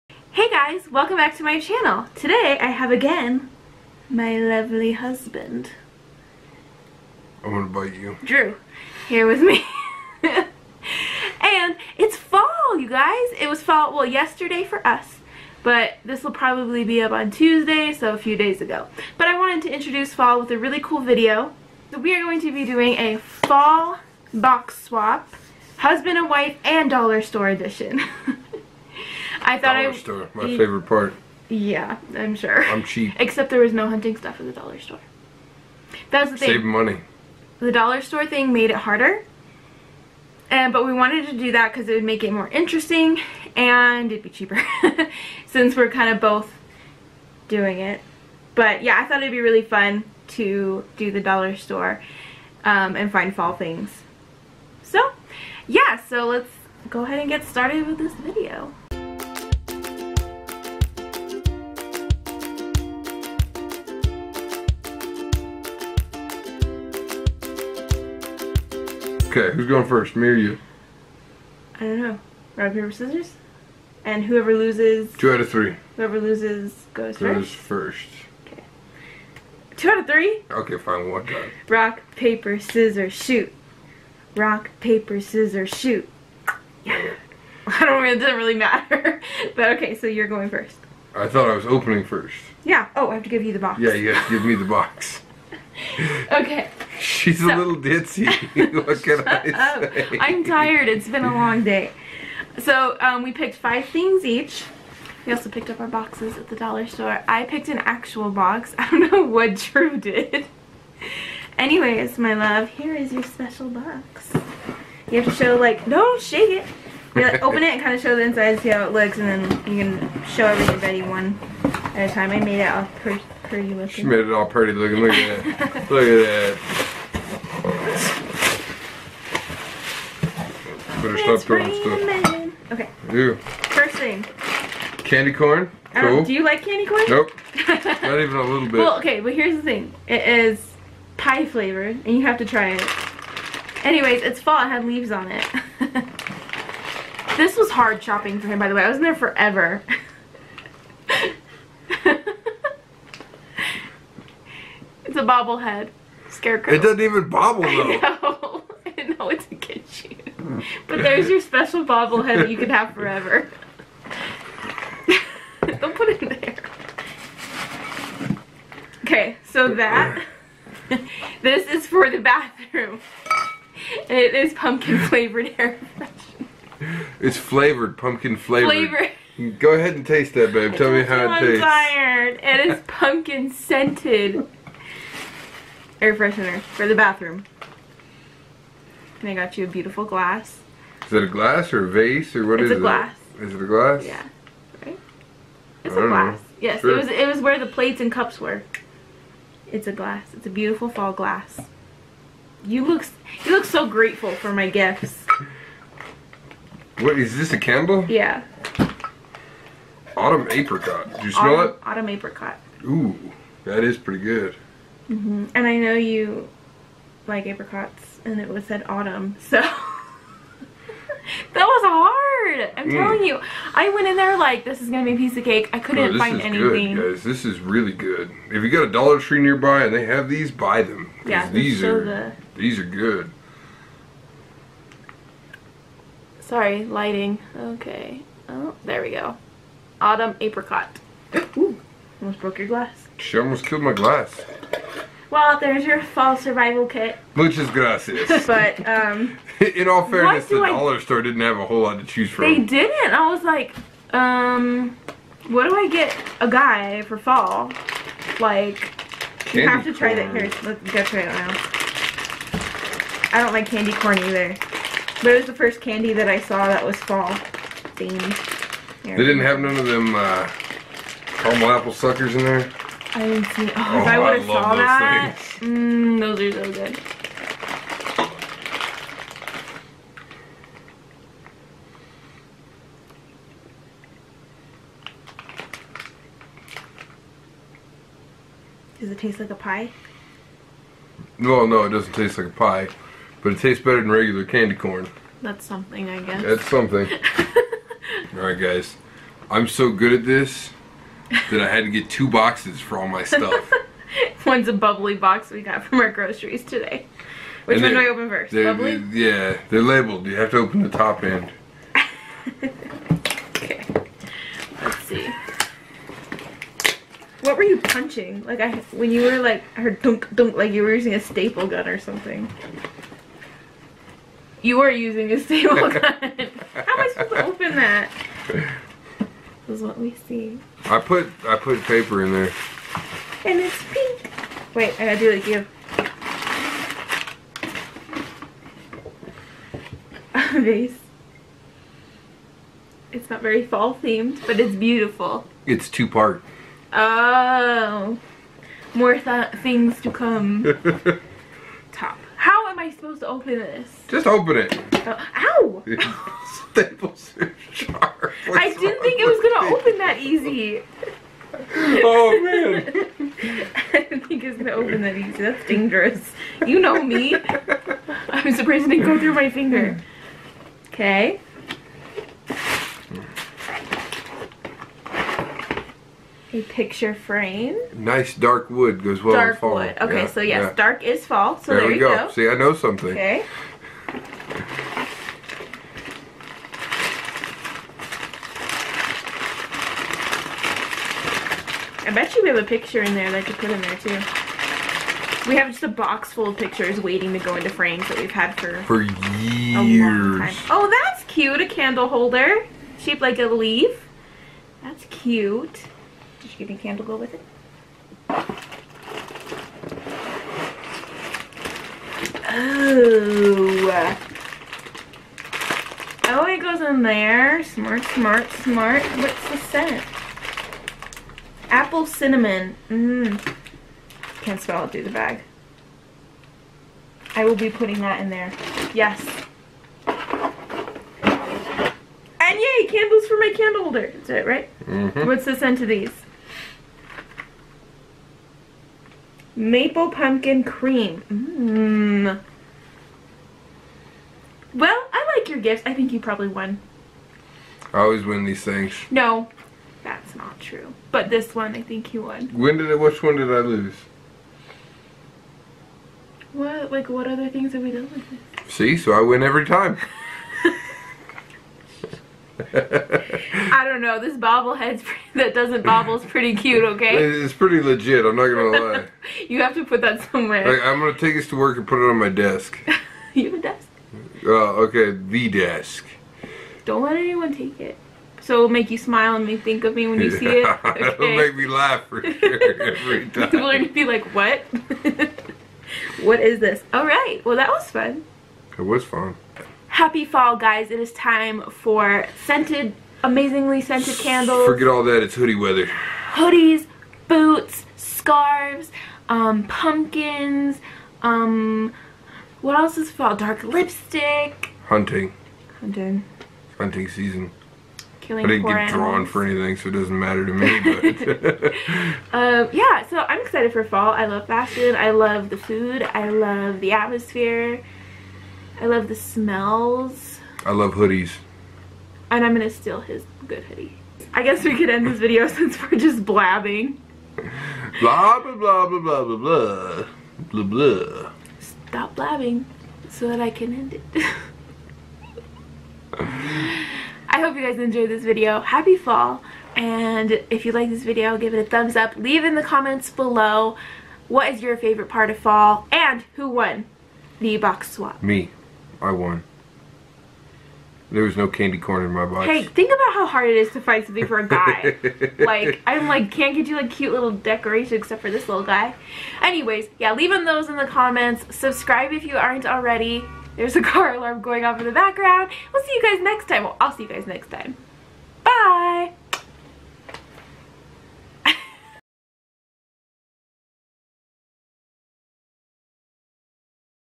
Husband And Wife Fall Box Swap (Dollar Store)
Happy Fall! We had so much fun making this video and giving each other some fall gifts!

***SOCIAL MEDIA***